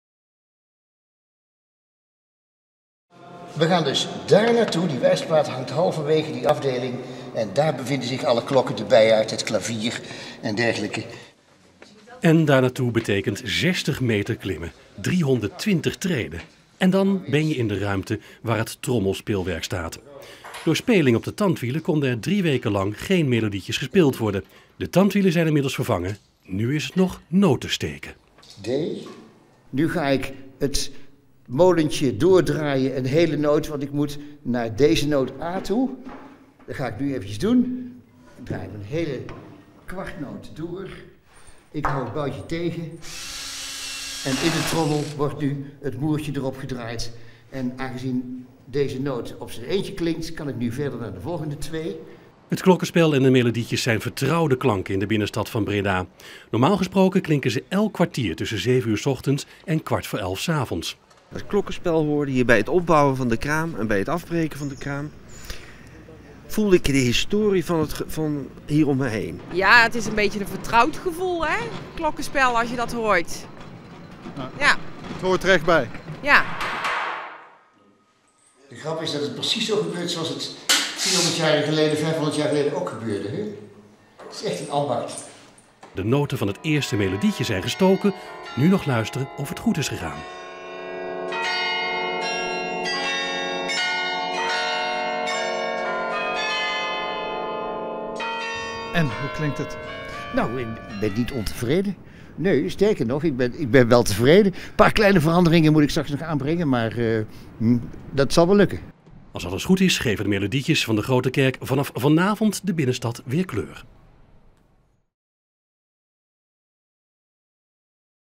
Grote Kerk Breda maakt weer muziek
- Drie weken lang is ze stil geweest, de Grote Kerk in het centrum van Breda. Sinds 9 november klonken er geen vrolijke riedeltjes meer uit het carillon en was de klok die de uren aangeeft, stil.